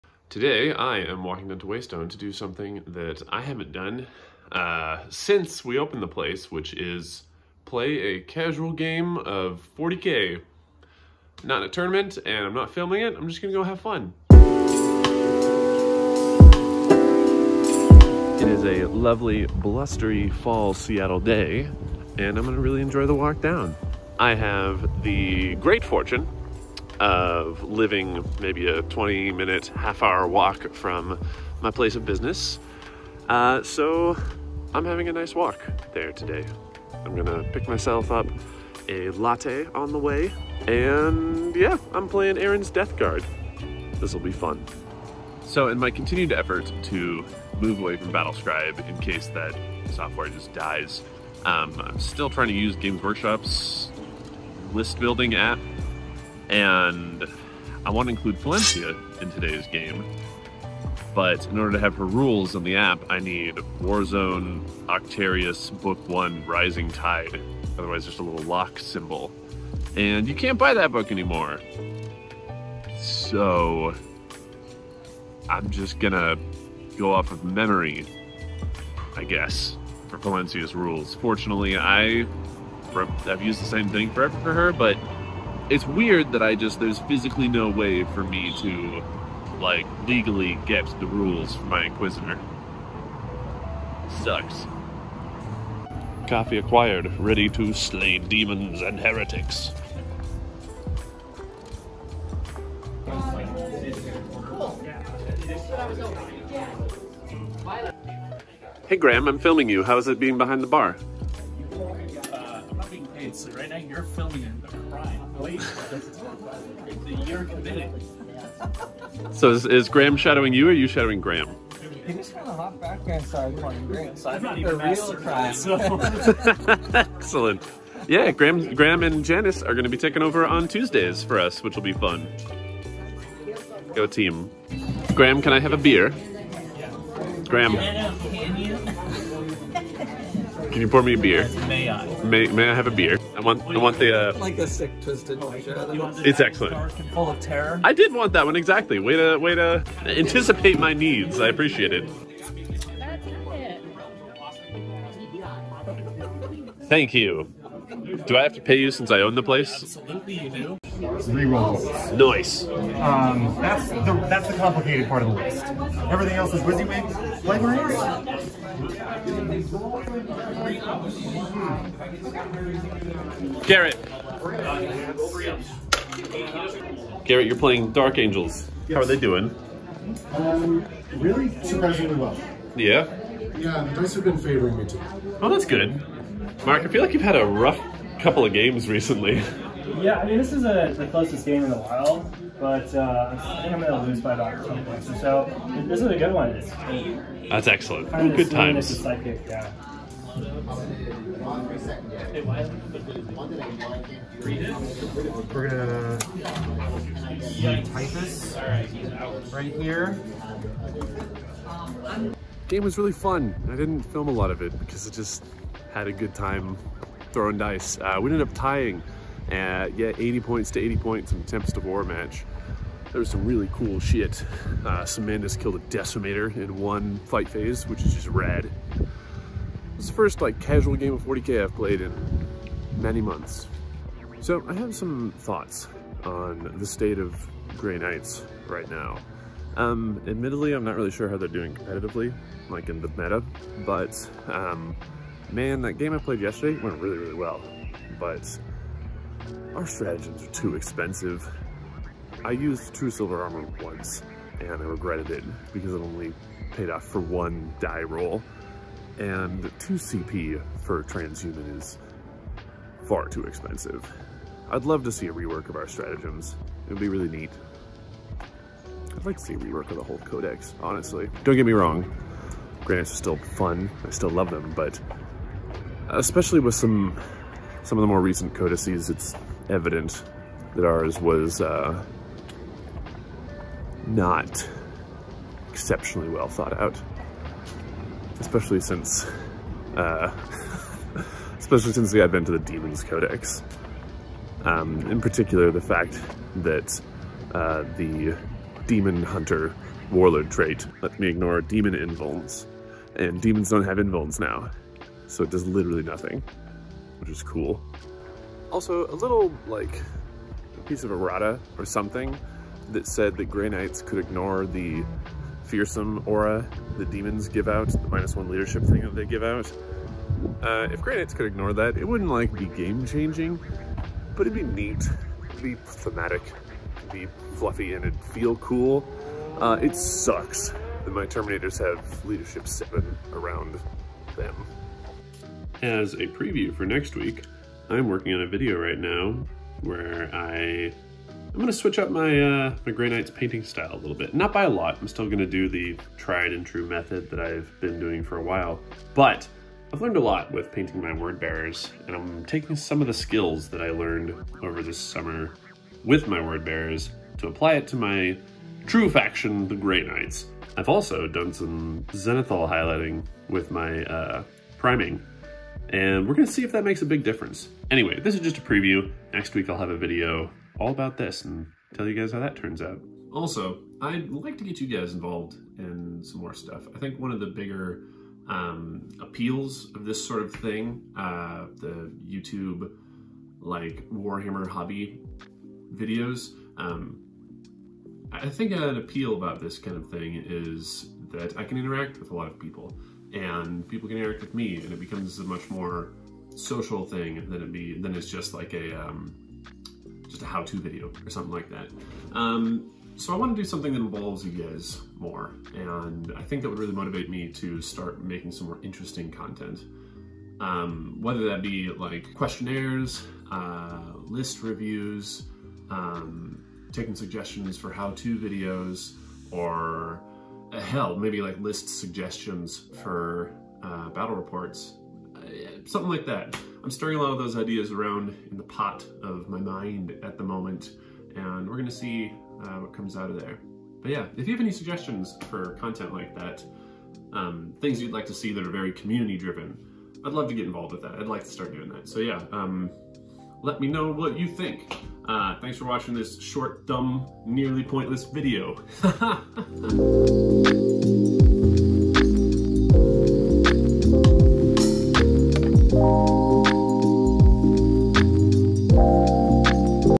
A Day Out with my Grey Knights
On rare occasion I am afforded the opportunity to play a game of Warhammer 40,000 without the camera rolling.